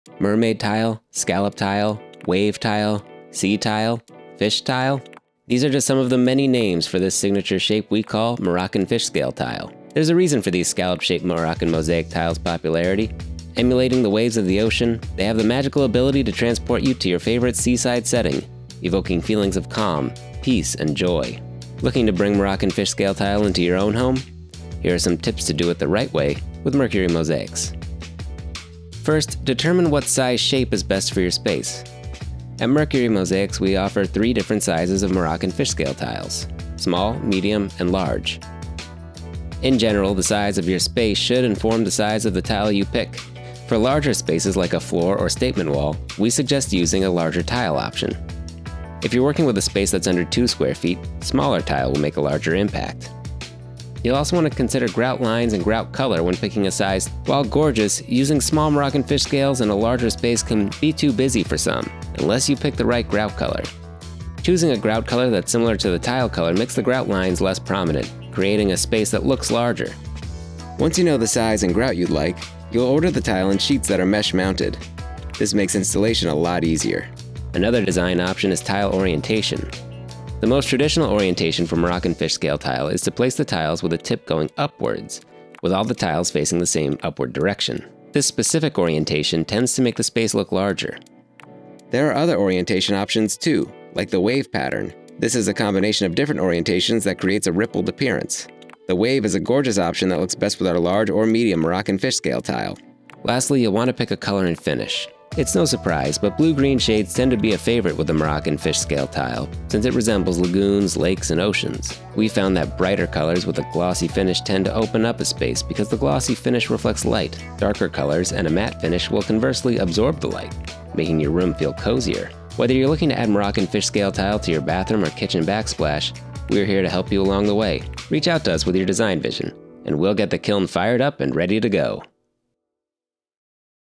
How to Fishscale Your Home (the Moroccan Tile Way)
It’s no wonder why Moroccan fish scale tile is such a popular option: By emulating your favorite ocean vista, it has the power to evoke feelings of calm and joy. Whether you’re looking to add Moroccan fish scale tile to your bathroom or kitchen backsplash, we have a few pointers to help you come up with the perfect tile design. Here, we break down helpful tips for picking everything from tile size, color and orientation.

Ready to get started? Head over to our selection of Moroccan Fish Scale Tile and let’s get designing